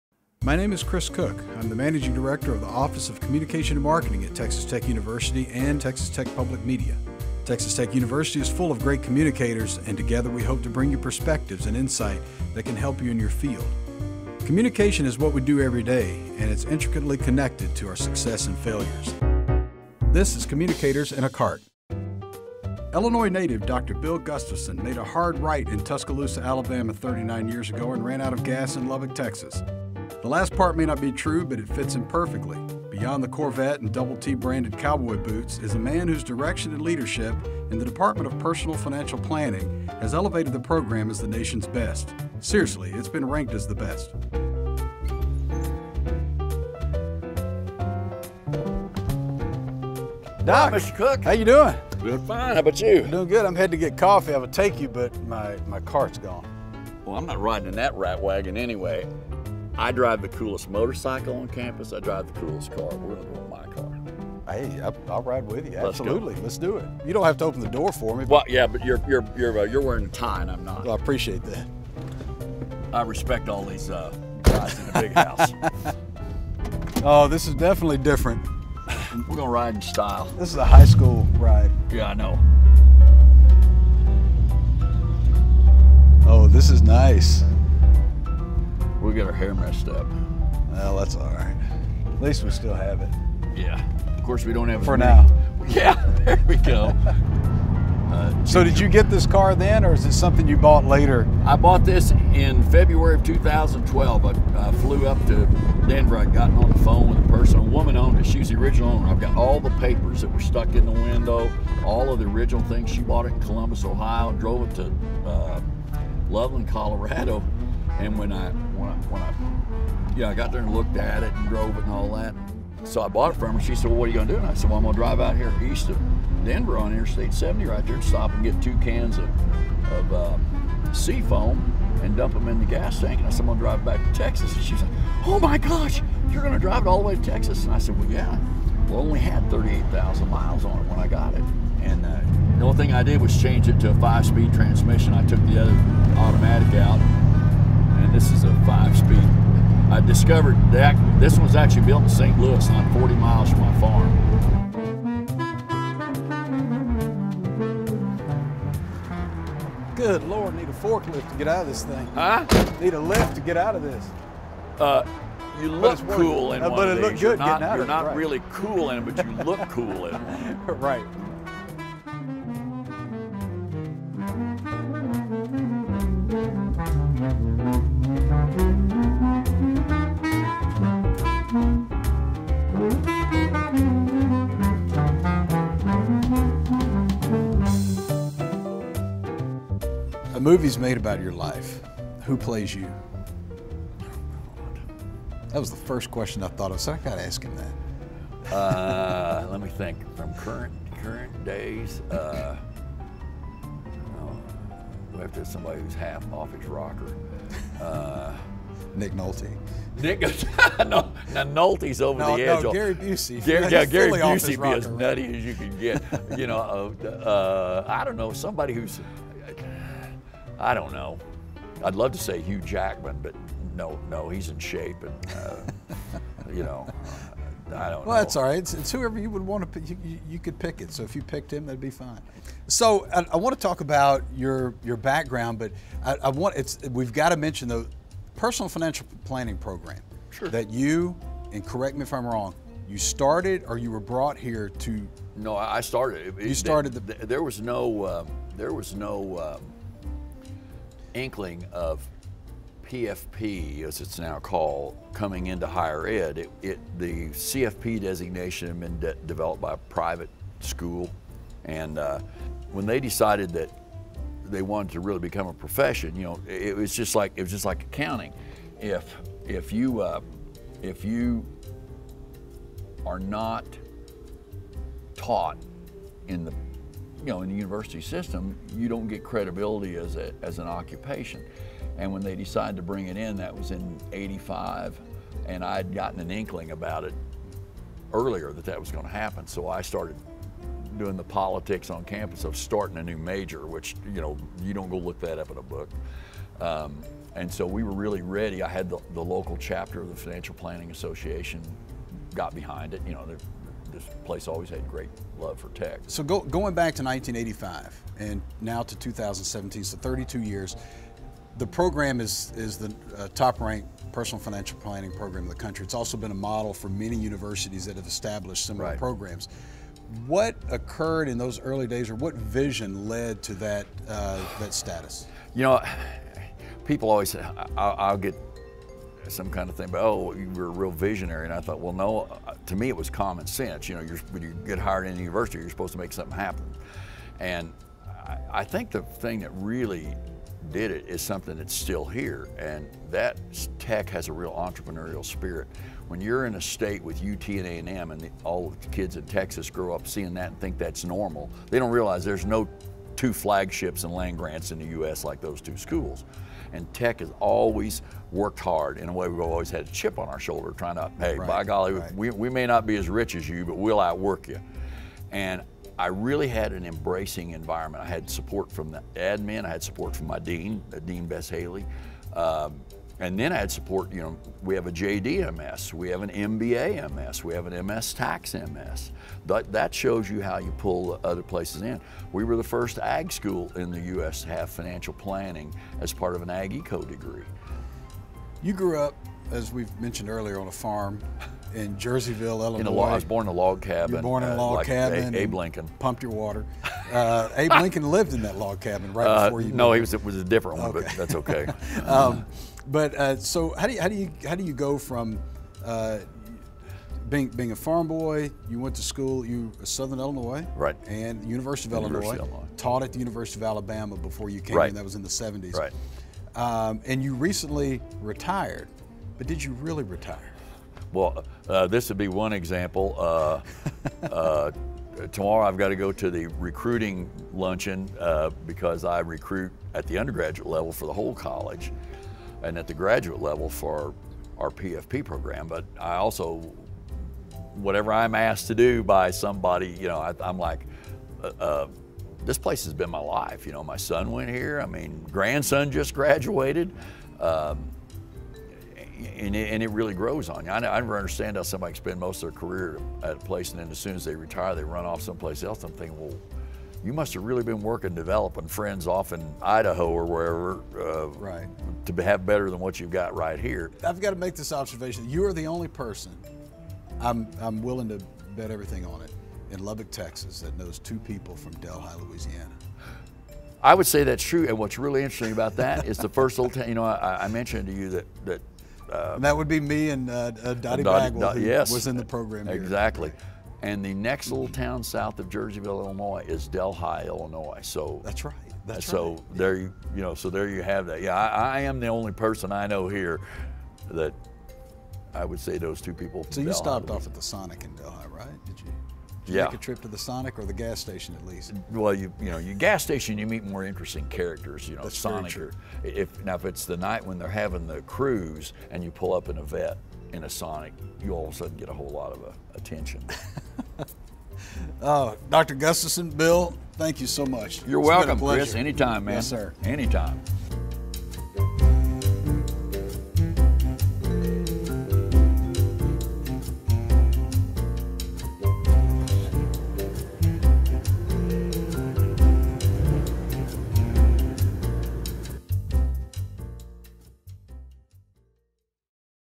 Dr. Bill Gustafson | Communicators In A Cart
Dr. Bill Gustafson started the Personal Financial Planning Program at Texas Tech, which is now ranked as the best. Find out how he did it!